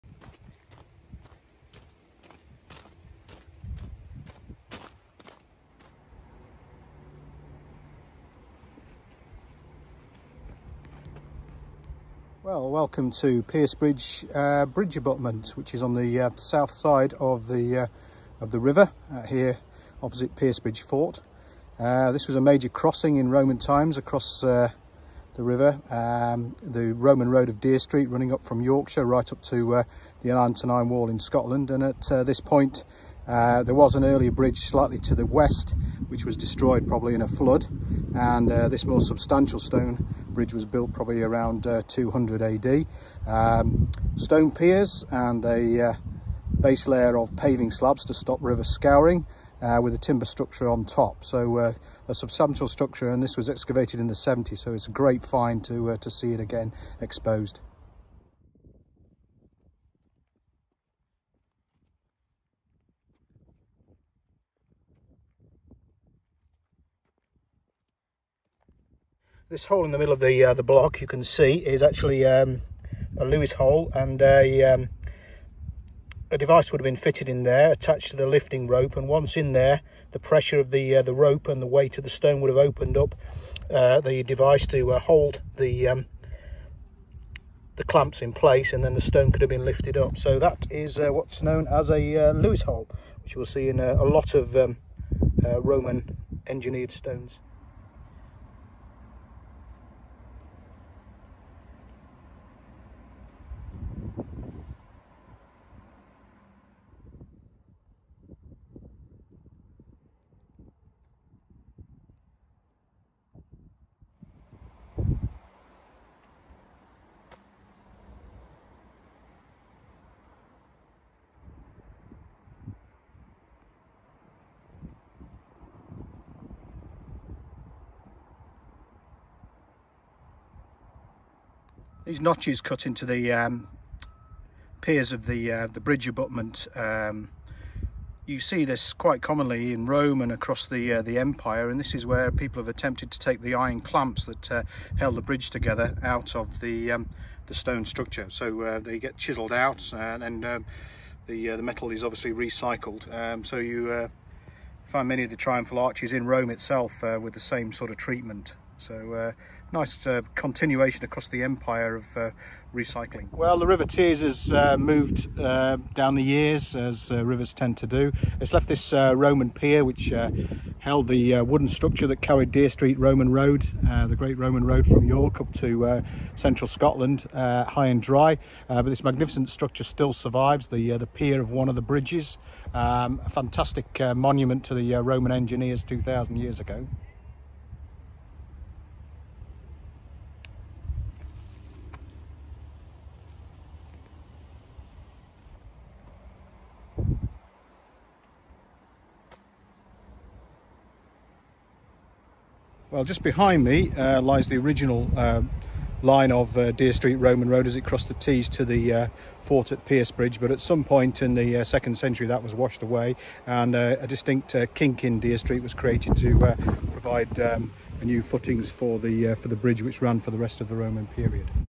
Piercebridge roman bridge
A quick tour of the roman crossing of the river Tees which carried Dere Street roman road north